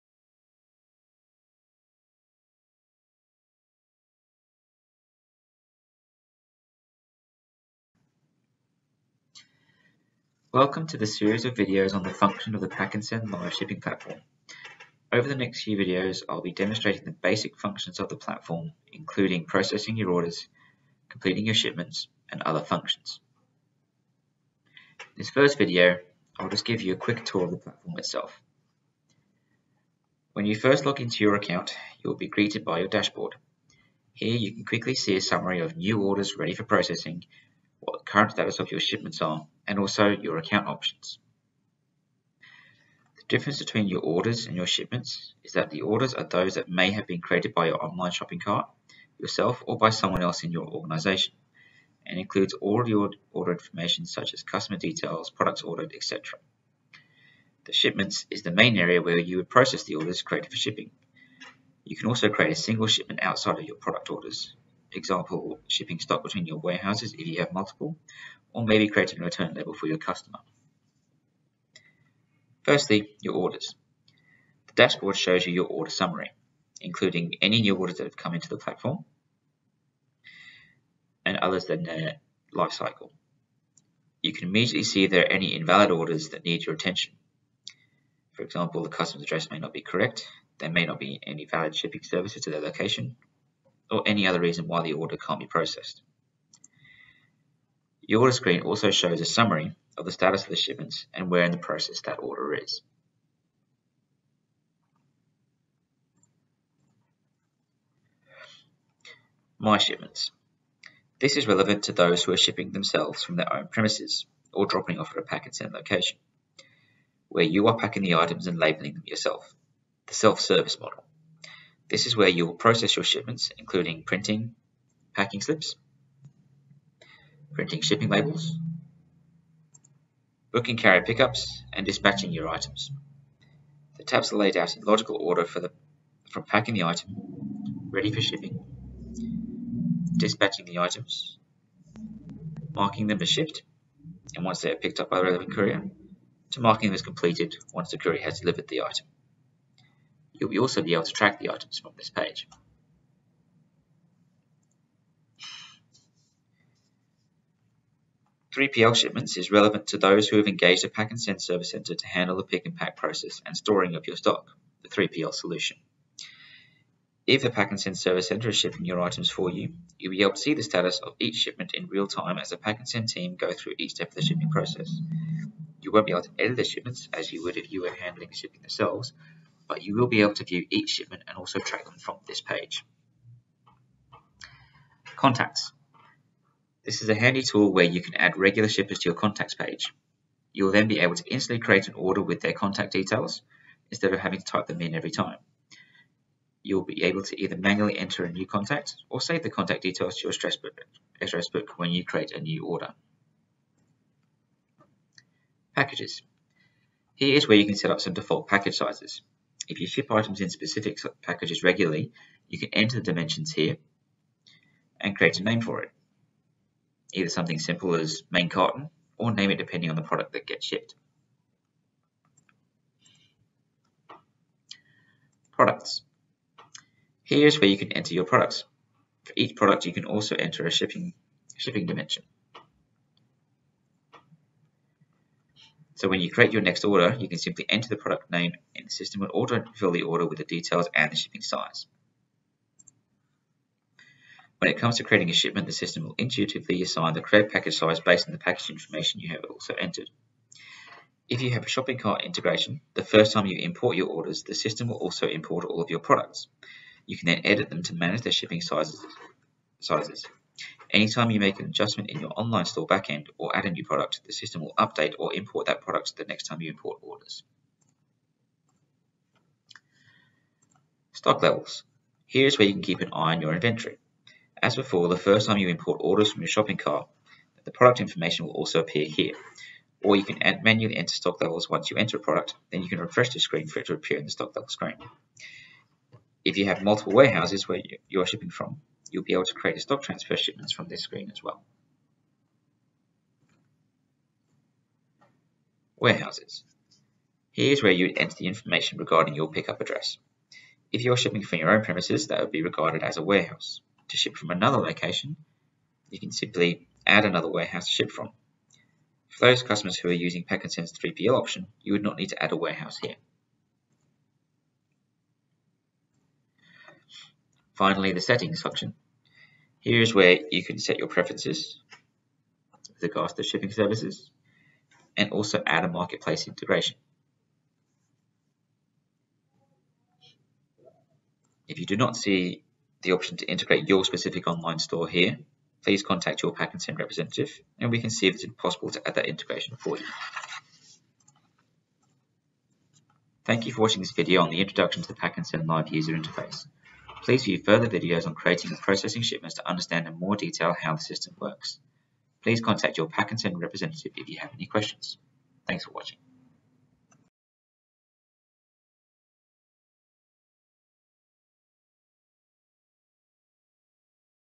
PACK & SEND Live - Intro to the Shipping Platform
Take a quick tour of the PACK & SEND Live Shipping Platform. In this video we highlight key areas from your dashboard and the key features of the platofrm.

If you are a regular sender and you need to scale up your retail business or pivot to an online store... you'll require a software platform to help superpower your ecommerce logistics!

PACK & SEND Live easily integrates with all leading eCommerce Shopping Platforms. Backed by our no cost tech and our live, local phone support with PACK & SEND, we have the tools and resources to help you accelerate your business.